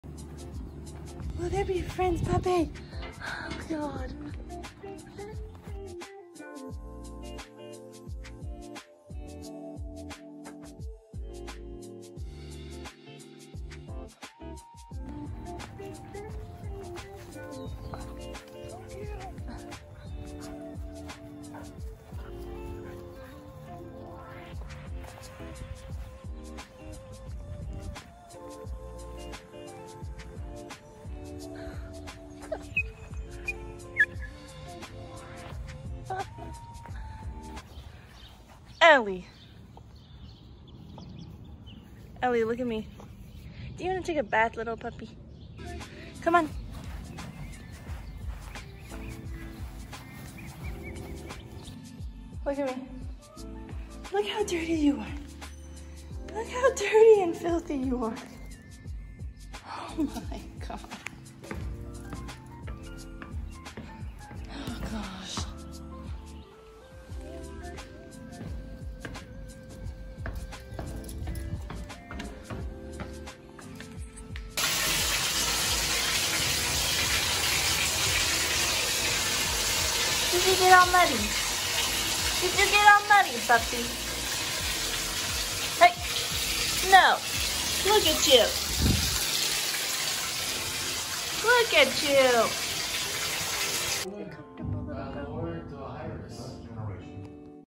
Lemon Beagle Gets Dirty in the Mud
Elly discovers mud for the first time with two big dogs who taught her to play dirty. A bright white beagle covered in mud. A bath was needed after her time in the dog park.

Follow Elly on Instagram: @thelemonybeagle